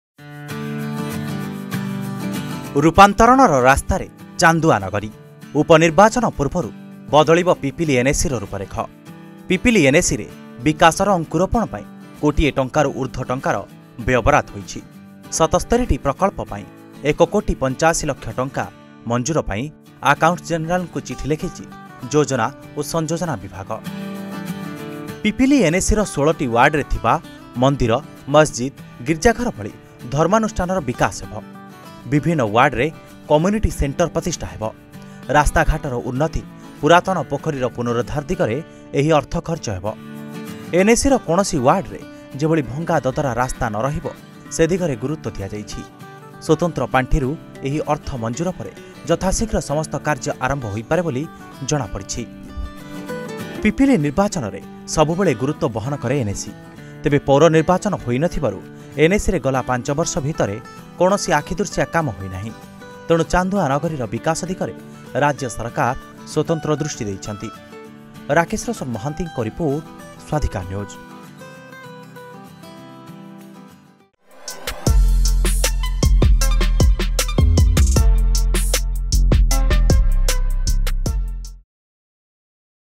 ରୁପାନ୍ତରଣର ରାସ୍ତାରେ ଚାନ୍ଦୁଆ ନଗରୀ ।
ରୁପାନ୍ତରଣର ରାସ୍ତାରେ ଚାନ୍ଦୁଆ ନଗରୀ । ଉପନିର୍ବାଚନ ପୂର୍ବରୁ ବଦଳିବ ପିପିଲି ଏନଏସିର ରୁପରେଖ